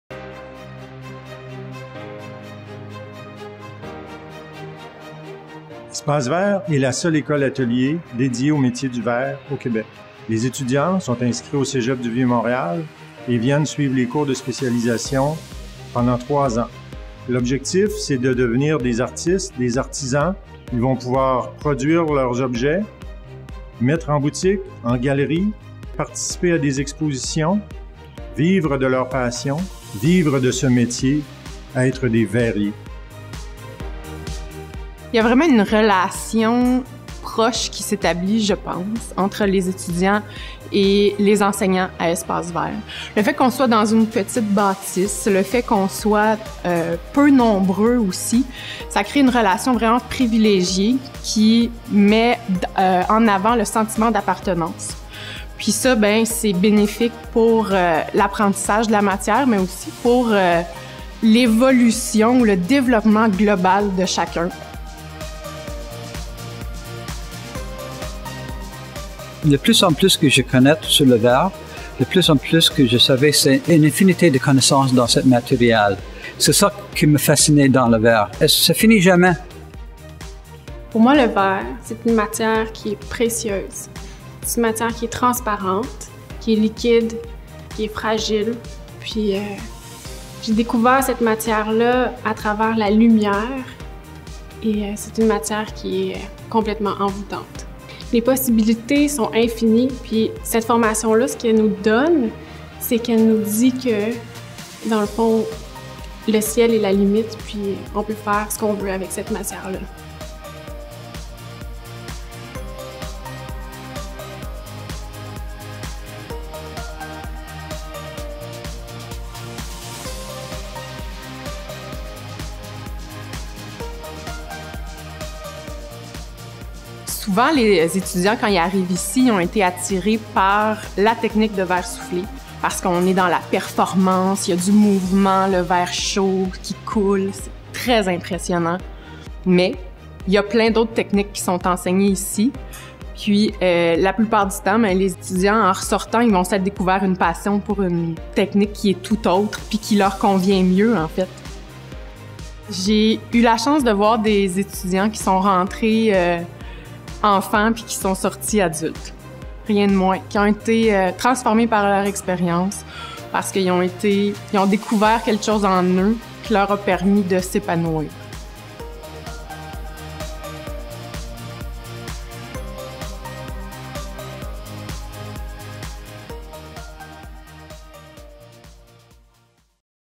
Techniques de métiers d'art, spécialisation verre - version longue
Espace Verre, la seule école-atelier d’art verrier au Québec
Programme technique en collaboration avec le cégep du Vieux Montréal